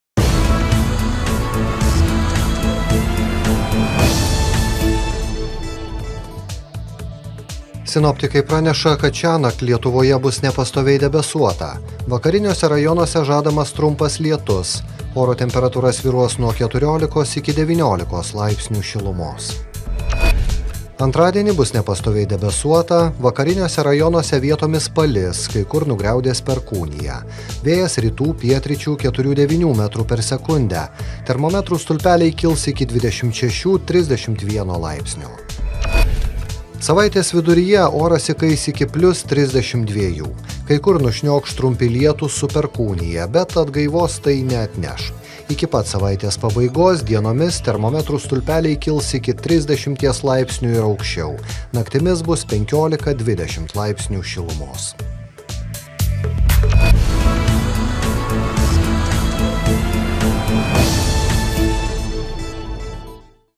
Po alinančios kaitros sulauksime atokvėpio: žadama perkūnija
Artimiausiomis dienomis sugrįš kaitra, lietaus debesys telksis arčiau jūros. Rugpjūčio 23 d. daugiausia vakariniuose rajonuose vietomis trumpi lietūs, perkūnija. Vėjas rytinių krypčių, 4–9 m/s, per perkūniją kai kur gūsiai 15 m/s. Temperatūra naktį 14–19, dieną 26–31, pajūryje apie 25 laipsnius.